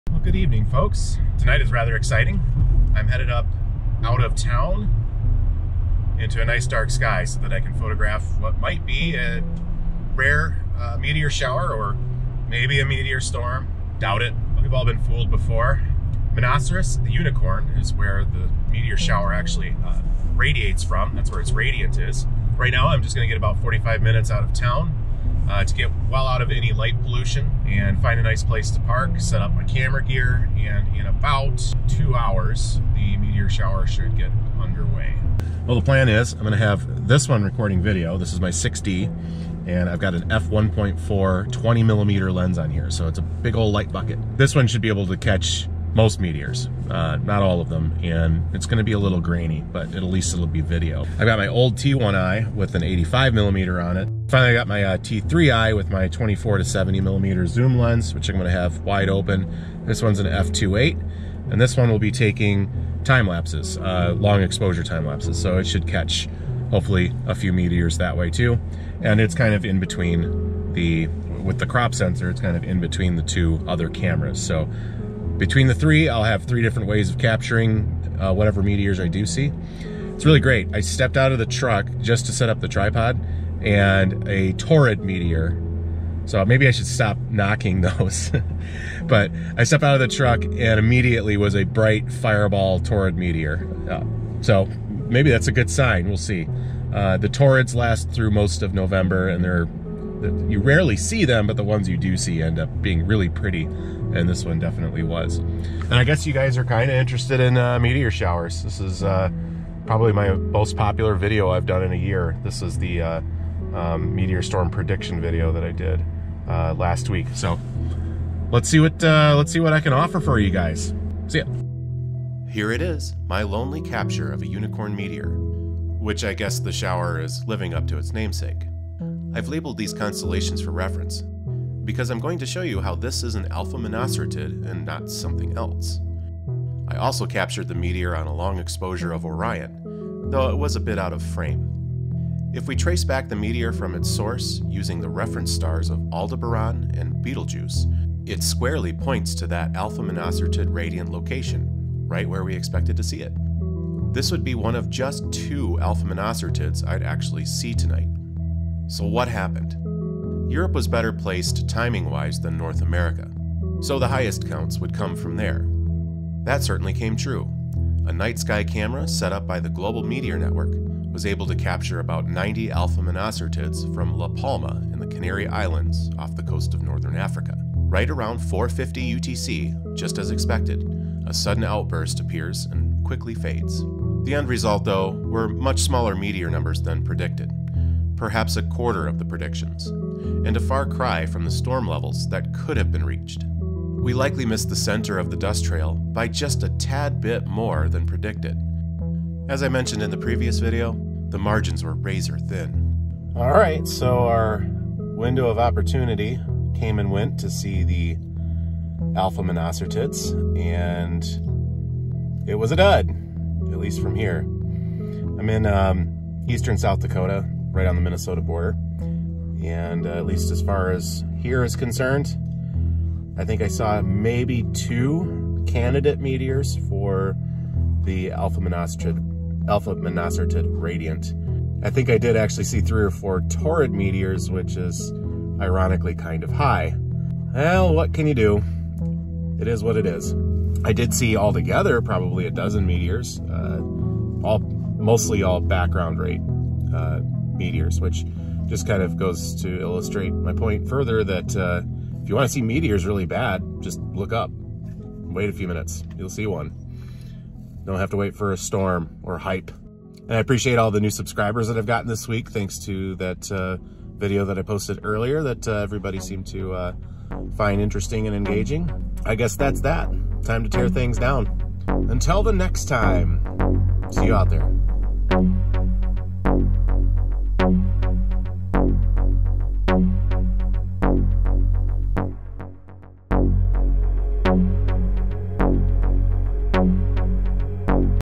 A Unicorn Meteor. Alpha Monocerotids 2019. November 21 04:50 UTC
Skip to 2:00 for footage/capture. So the meteor shower came and went. How did it go? Well for me, just 2 meteors spotted from this shower. And I captured one on two cameras!

How I know its an Alpha Monocerotid and not something else.

Hopefully this is a learning experience for all of us.  Peter and Esko learned something about the position of the dust trail. We learned not to hype things based on hopes and conjecture and hopefully you learn how to spot which parent radiant belongs to which meteor! I break that down here.  I film one realtime meteor and also capture one in a long exposure near orion.

Alpha Monocerotid metoer shower captured with a modified Canon T3i, and stock Canon 6D mkII.

"Unicorn Meteor Storm" didn't live up to the expectations.  Unicorn Meteor Shower 2019 was underwhelming. Realtime Meteor capture of an Alpha Monocerotid during "Unicorn Meteor Shower"